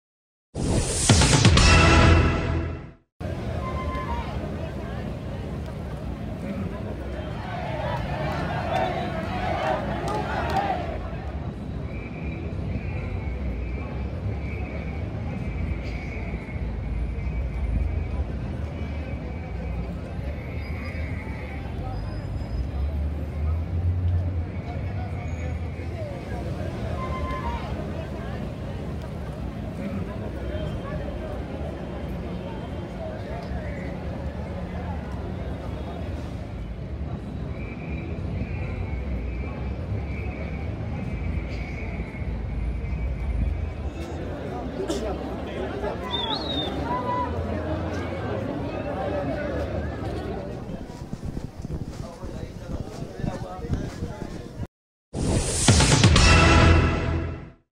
👉Alan García: formaron largas colas para despedirse
Imágenes aéreas de las largas colas de personas que se formaron en las inmediaciones de la sede del Partido Aprista Peruano para despedir al difunto expresidente Alan García, quien falleció el pasado miércoles 17 de abril.